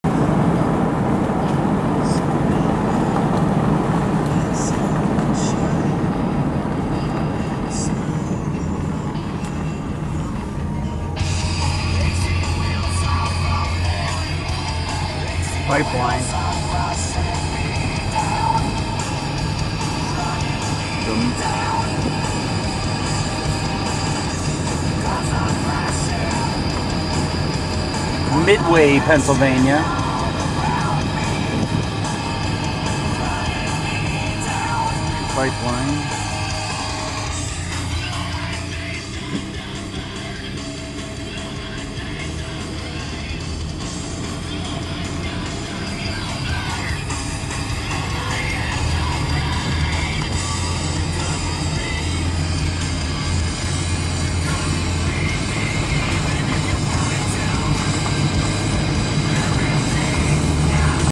Pipelines in Midway PA
Natural gas pipeline construction in Midway, PA - video shot on 12/24/2012